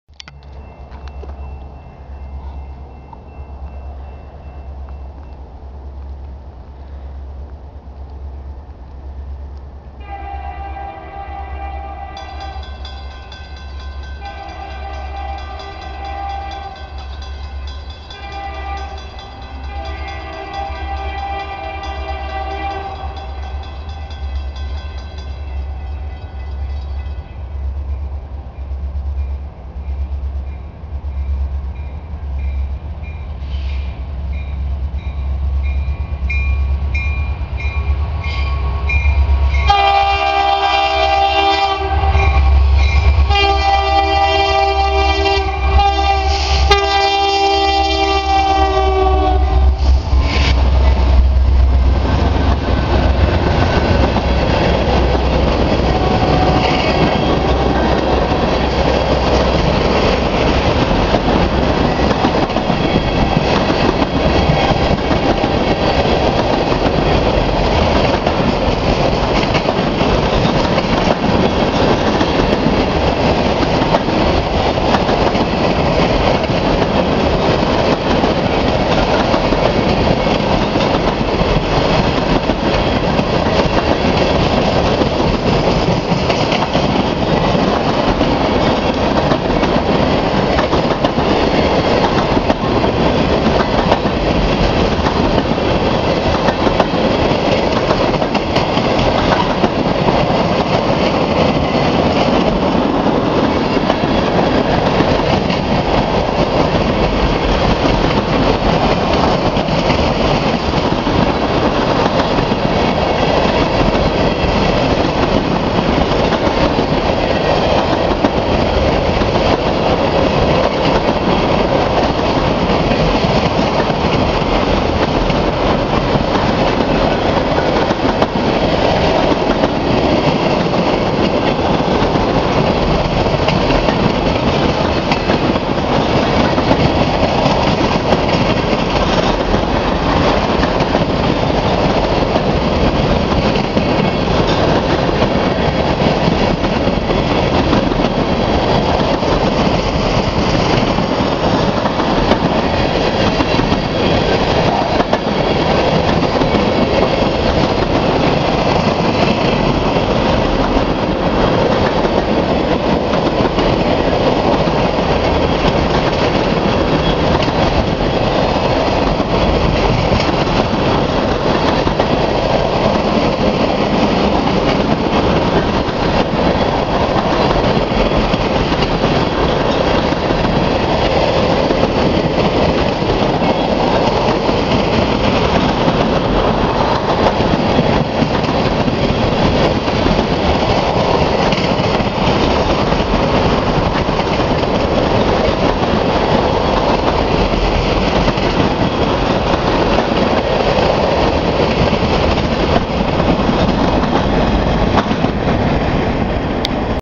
BNSF Leads on 65D
BNSF (9624) SD70MAC leads, with two NS engines trailing, hauling empty tank cars (65D).